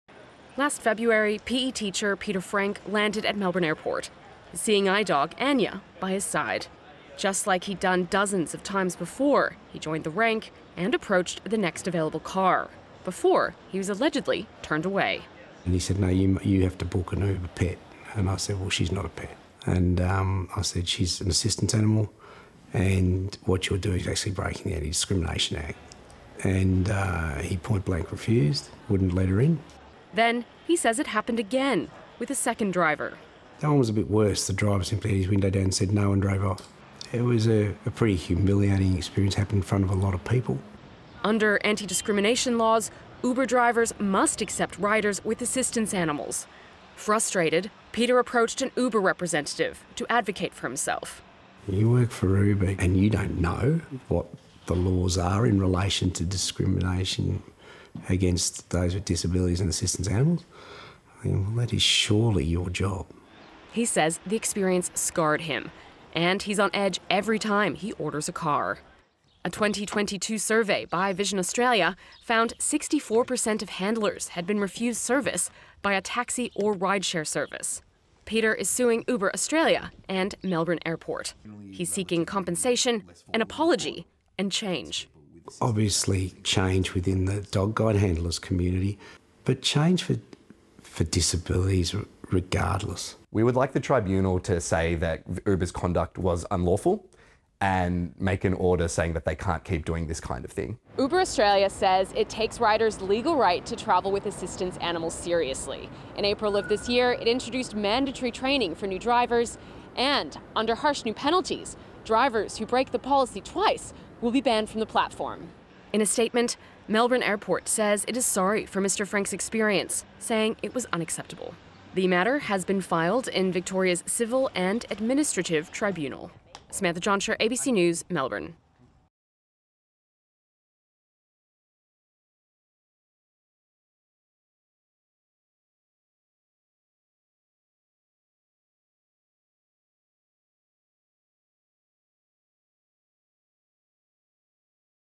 Man sues Uber and Melbourne Airport after being denied service | ABC News
Melbourne teacher Peter Frank and his assistance dog are seeking compensation and "change" after two drivers refused to pick him up at Melbourne Airport in 2023.